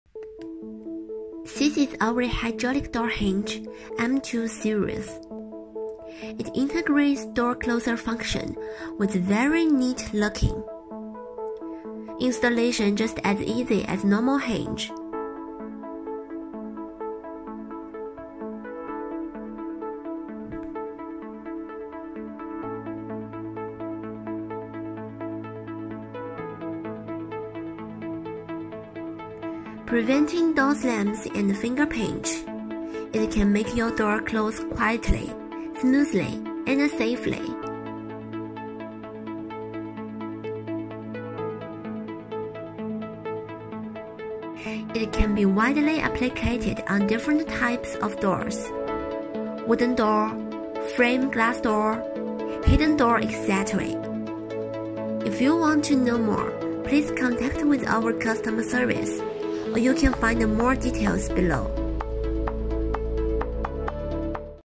Hydraulic door closer hinge, anti pinch and prevent door slams
Ningbo Pentagon Damper Corporation specializes in making innovative hydraulic door hardware.
M2 hydraulic hinge is one of the main product integrates door closer  and door stopper functions in door hinge.